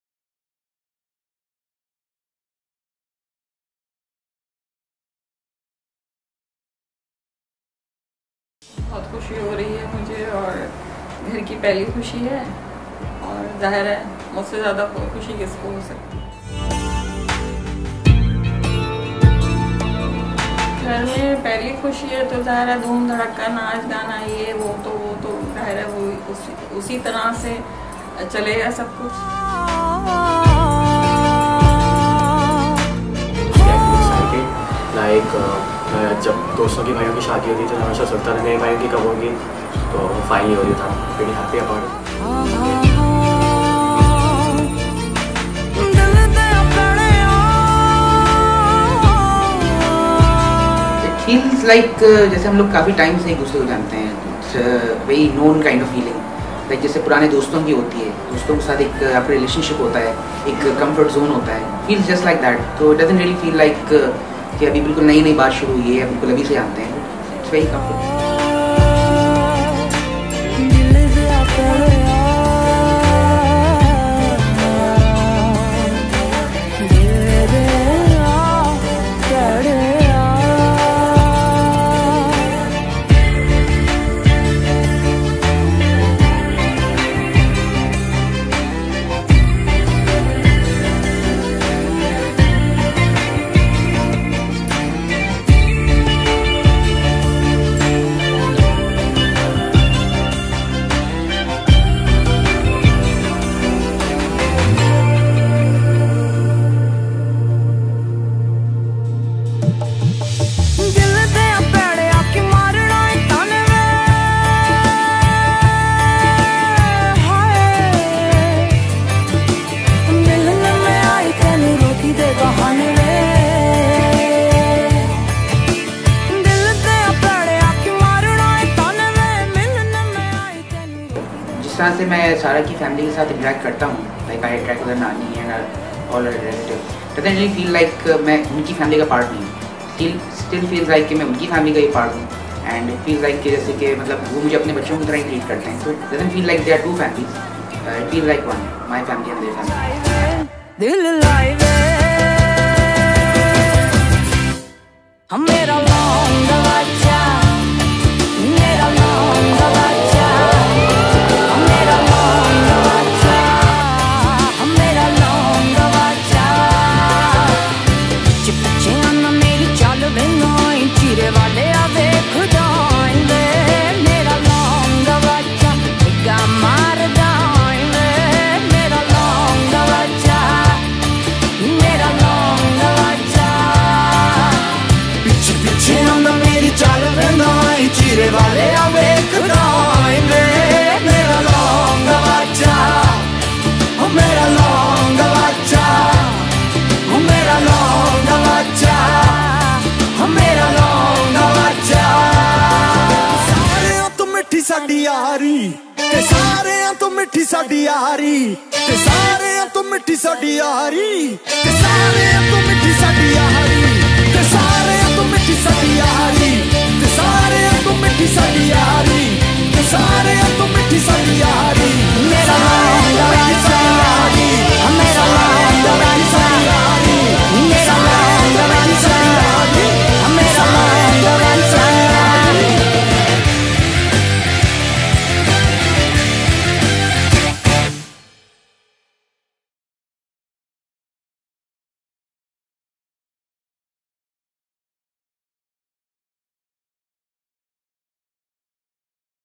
Cinematic Memorable Highlights of Talal & Sarah | Pakistani Wedding Films
Talal and Sarah's Engagement '18 - Watch the rings get exchanged as the first of the many lovely moments we are yet to capture of this couple!

Copyright©: Nyala Arts 2018